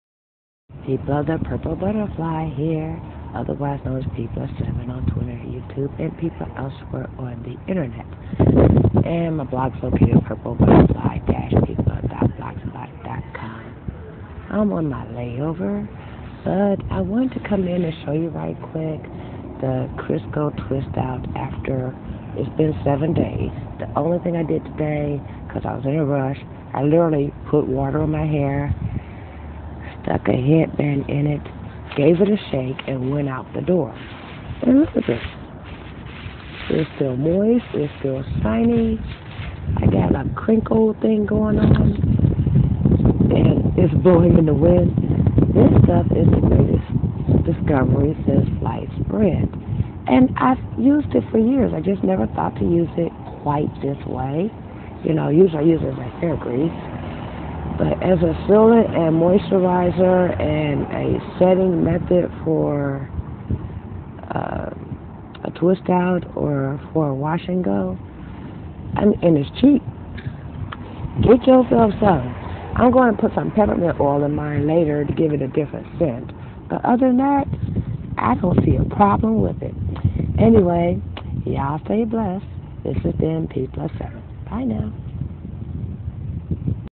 299. Crisco "butter" after 7 days
7 days later and hair is still responding positively to crisco as a "butter" application. If you are allergic to shea butter, this is a great substitute.